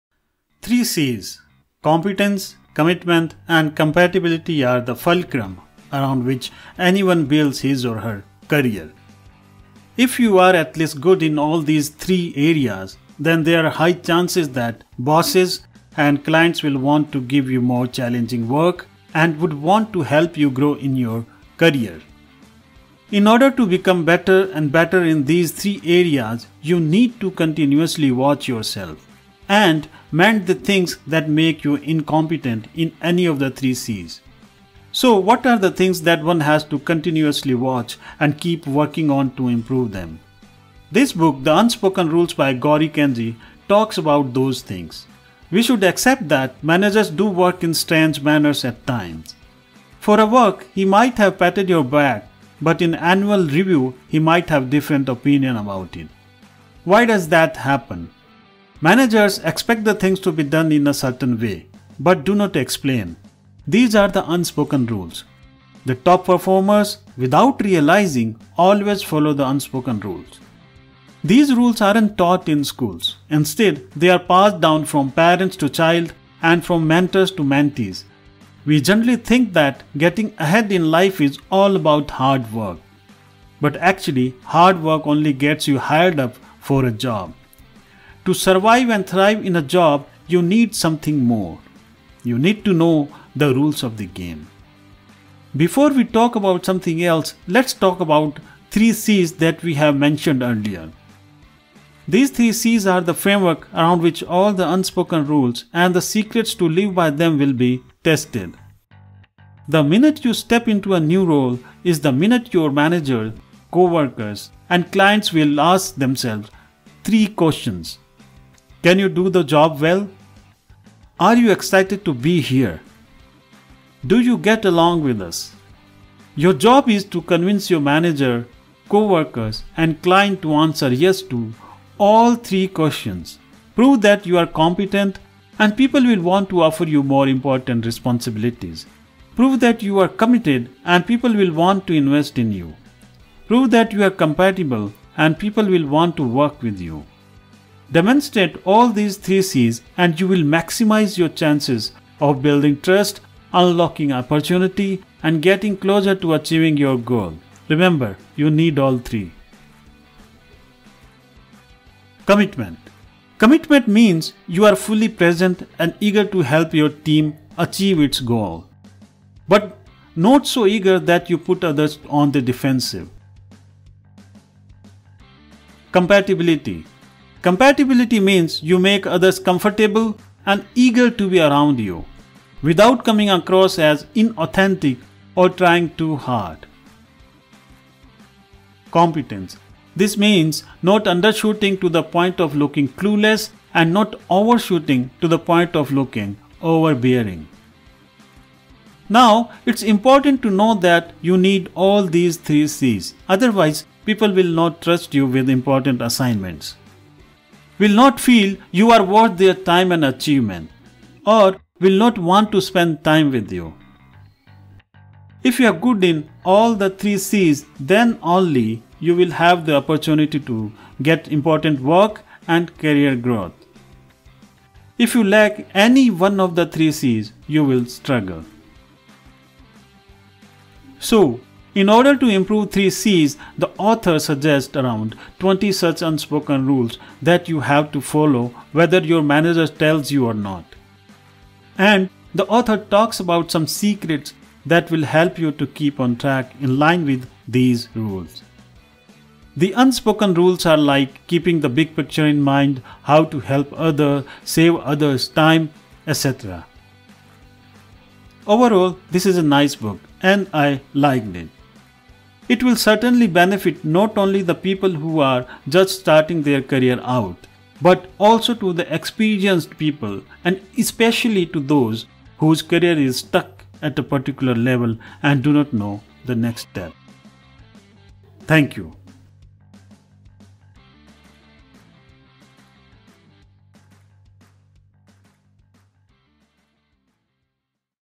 The Unspoken rules By Gorick NG | A Book Review
3 C's - Competence, Commitment and compatibility are the fulcrum around which any one builds his or her  career. If you are at least good in all these three areas then there are high chances that bosses and clients will want to give you more challenging work and would want to help you grow in your career. In order to become better and better in these three areas you need to continuously watch yourself and mend the things that make you incompetent in any of the 3 Cs. So what are the things that one has to continuously watch and keep working on to improve them ? This book The Unspoken Rules by Gorick NG talks about those things. We should accept that Managers do work in strange manners at times. For a work he might have patted your back but in annual review he might have different opinion about it. Why does that happen ?

Managers expect the things to be done in a certain way but do  not explain,  these are the unspoken rules. The top performers,  without realizing,  always follow the unspoken rules. These rules aren’t taught in schools. Instead they are passed down from parents to child and from mentors to mentees. We generally think that getting ahead in life is all about hard work but actually hard work only gets you hired up for a job. To survive and thrive in a  job you need something more, you need to know the rules of the game. Before we talk about something else let’s talk about three C’s that we have mentioned earlier.These three C’s are the framework around which all the unspoken rules and  the secrets to live by them will be tested.

The minute you step into a new role is the  minute your managers, coworkers, and clients will ask themselves three questions -
“Can you do the job well ?”
“Are you excited to be here ? ”
“Do you get along with us ?”

Your job is to convince your manager, coworkers, and clients to answer “Yes !”  to all three questions. Prove that you are competent, and people will want to offer you more important responsibilities. Prove that you are committed, and people will want to invest in you. Prove that you are compatible, and people will want to work with you. Demonstrate all these three C’s and you will maximize your chances of building trust, unlocking opportunity, and getting closer to achieving your goal. Remember, you need all Three.

Commitment - Commitment means you are fully present and eager to help your team achieve its goals - but not so eager that you put others on the defensive.

Compatibility - Compatibility means you make others comfortable and eager to be  around you - without coming across as inauthentic or trying too hard.

Competence  - Competence means you an do your job fully , accurately, and promptly  without needing to be micromanaged - and without making others look bad.
Now, It’s important to know that you need all these three Cs . Otherwise, People will not trust you with important assignments, will not feel you are worth their time and investment, or will not want to spend time with you. If you are Good in all the three Cs then only you will have the opportunity to get important  work and career growth.  If you lack any one of the three Cs you will struggle.

So, In order to improve these 3 Cs the author suggests around 20 such unspoken rules that you have to follow whether your manager tells you or not. And, the author talks about some secrets that will help you to keep on track in line with these rules.

The unspoken rules are like - keeping the big picture in mind, how to help others , save others time etc .

Overall, this is a nice book and I liked it. it will certainly benefit not only the people who are just starting their career but also to the experienced people and especially to those whose career is stuck at a particular level and do not know the next step.